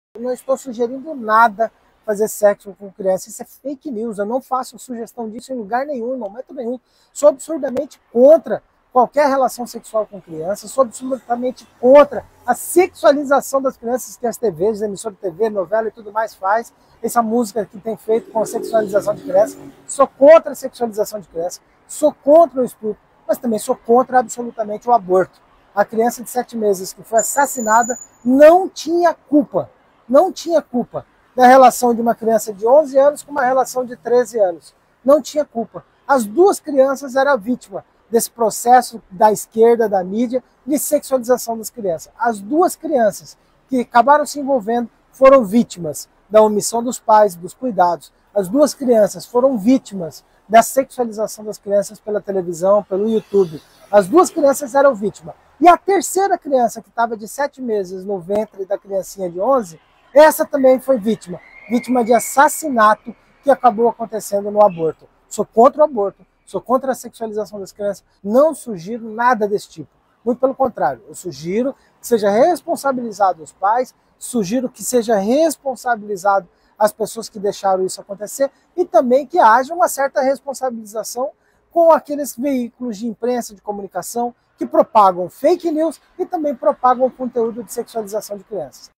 Sou ABSOLUTAMENTE CONTRA s3x0 com menores, estupro e aborto!
Não caia em fakenews, pegaram um recorte duma entrevista que se tratava do aborto de uma criança de 7 meses no ventre de outra criança de 11 anos, que foi gerada por uma relação com outra criança de 13 anos.

São três vítimas!!! As crianças de 11 e 13 anos, são vítimas da sexualização infantil, dos programas de TV da omissão dos cuidados dos pais. O bebê de 7 meses foi vítima de assassinato, pois abortaram uma criança já desenvolvida que não tinha culpa de nada.

Assunto é polêmico, mas não caia em fakenews, eu não relativizo o estupro, não sugiro ou incentivo sexo com menores, e todos sites de notícia que estão veiculando erradamente isso, serão processados.

Alguns dos sites que notificamos decidiram manter a matéria, pois alguém está pagando e estes interessados em me difamar preferem pagar a multa e esperar a justiça mandar tirar enquanto destroem minha reputação. Mas a justiça vai julgar isso também.

Enquanto esperamos a justiça, publico aqui minha opinião sobre o fato e peço que compartilhe pra me ajudar a espalhar a verdade.